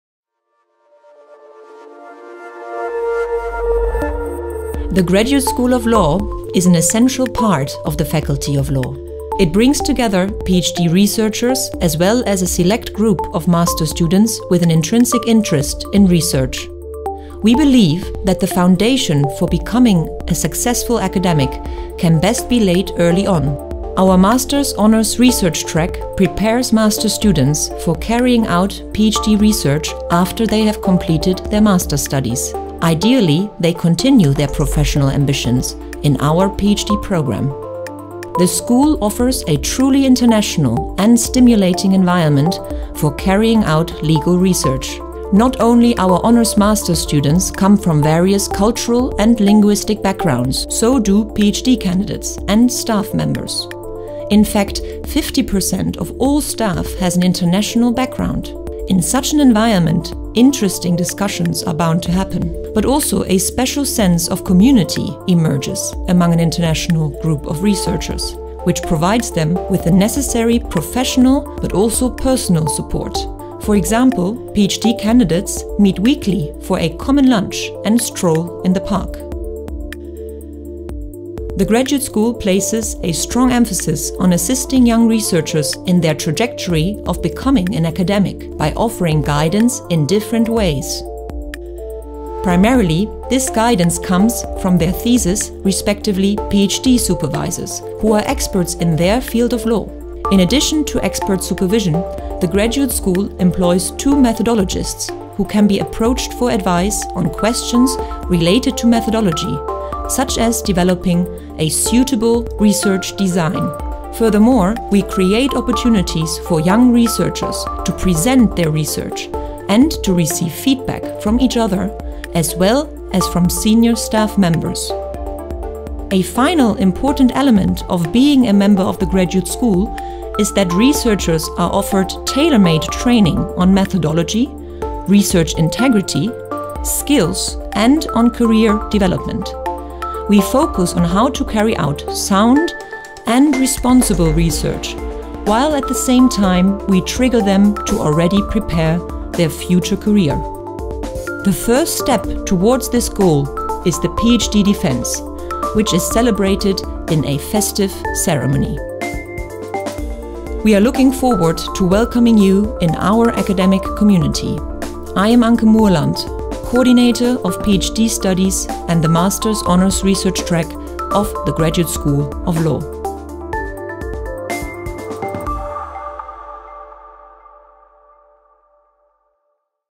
Maastricht Graduate School of Law
This video provides an overview of the activities of the Maastricht Graduate School of Law for PhD candidates and Master’s Honours Research Track students at the Faculty of Law, Maastricht University.